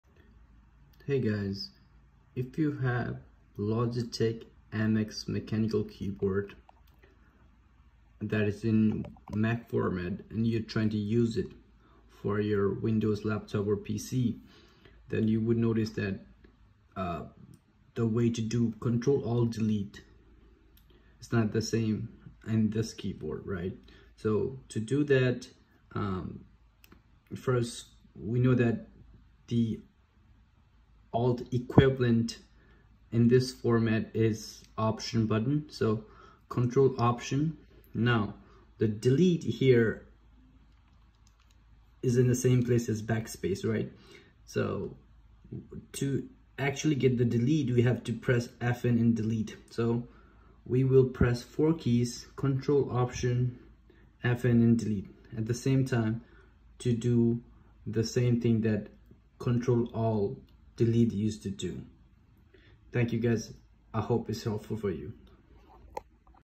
How to perform Control Alt Delete on a Logitech MX Mechanical keyboard?
If you have a Logitech Mac-formatted keyboard and you're unable to perform Control + Alt + Delete, then this video is for you.